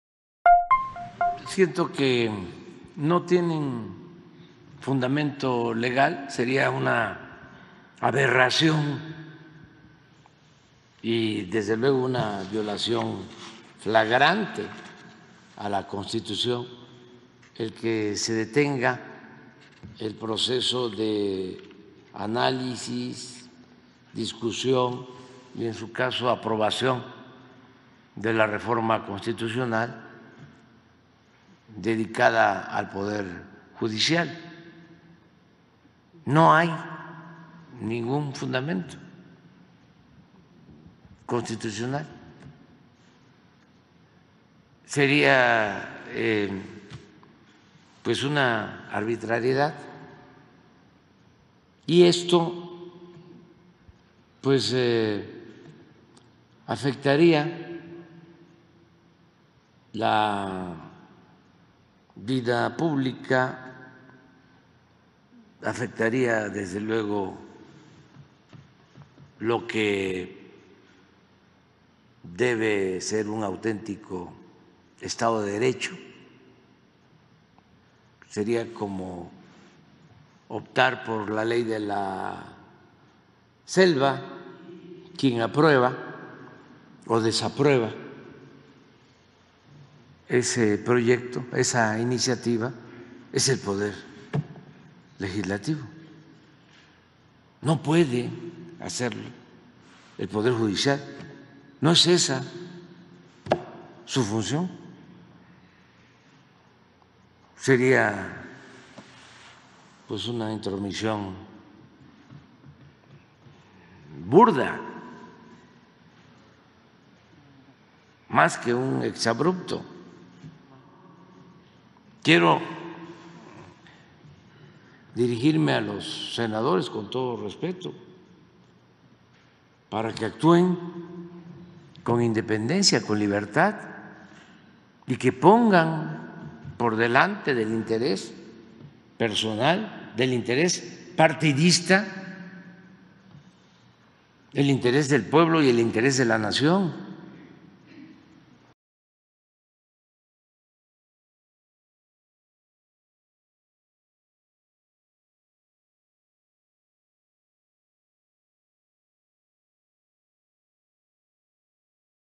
Sería aberración que SCJN frene reforma judicial.-AMLO
El Presidente consideró como aberración la posibilidad de que la Corte frene la reforma al Poder Judicial, señalando que afectaría la vida pública.